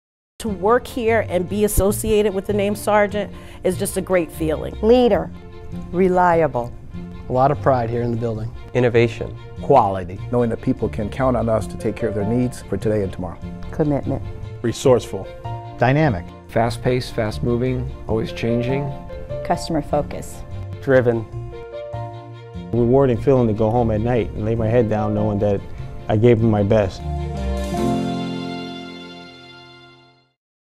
SARGENT Manufacturing Culture Video
Employees at SARGENT have strong brand loyalty, value their customers, and are proud to lead the industry in new product innovation.